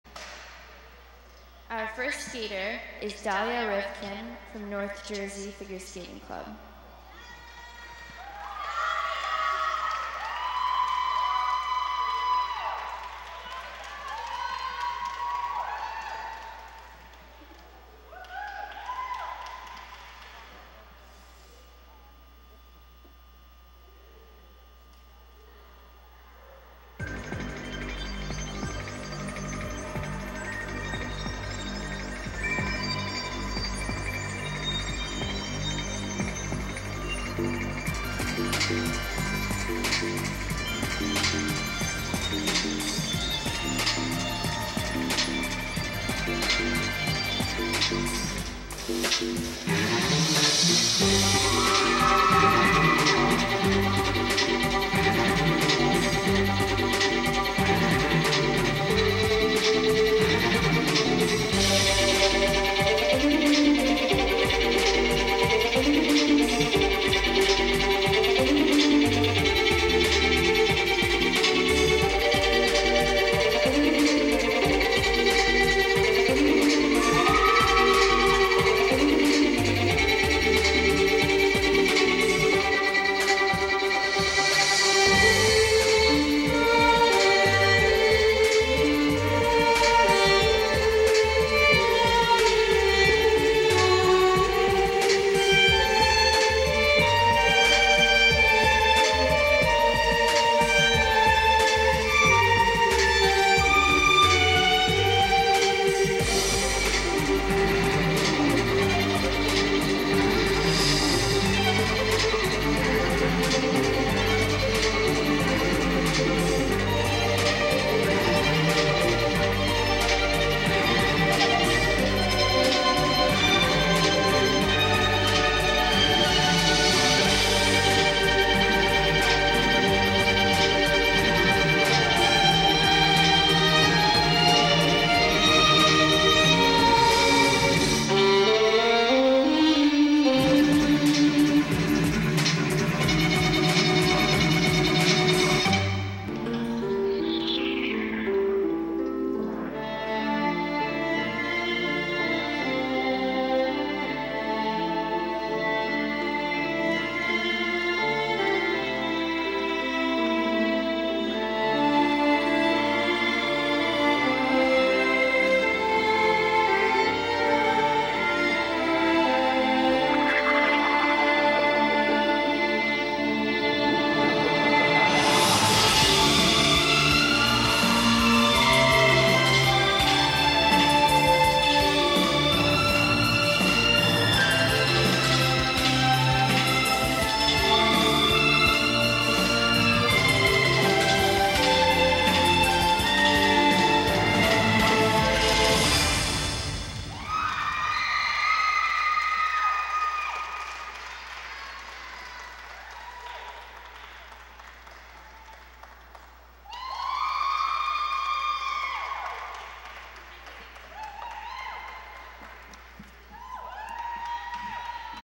Dalia Rivkin -- Lake Placid Championship (July 2012)
Dalia competing at the Lake Placid Championship (Junior Short) -- July 2012